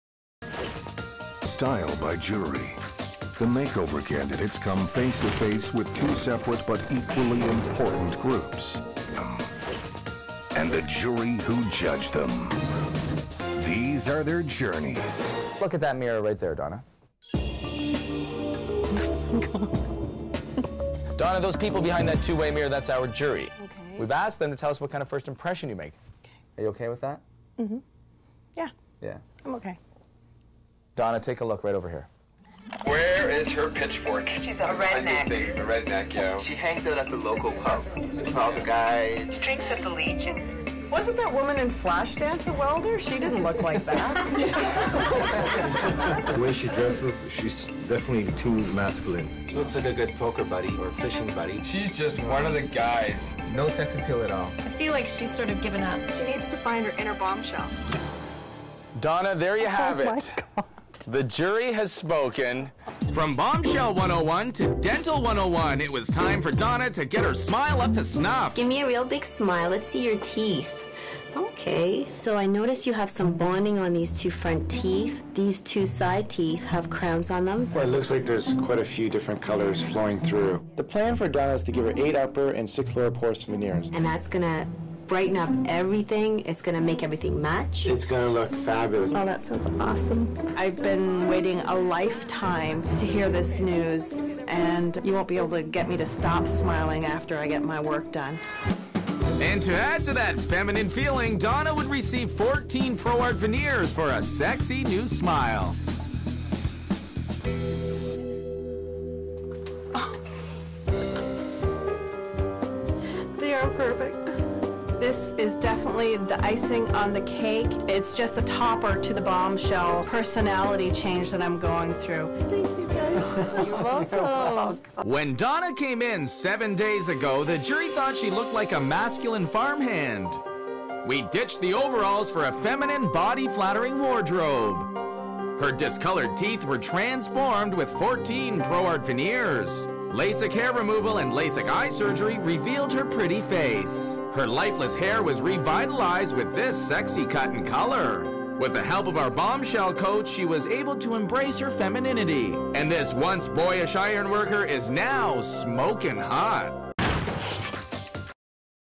Dental Bonding and Old Crowns Improved with Dental Veneers Toronto 122
Donna gets old dental bonding an old crowns replaced with dental veneers in Toronto.  She looks so much better with a new white smile 14 veneers and no more yellow teeth.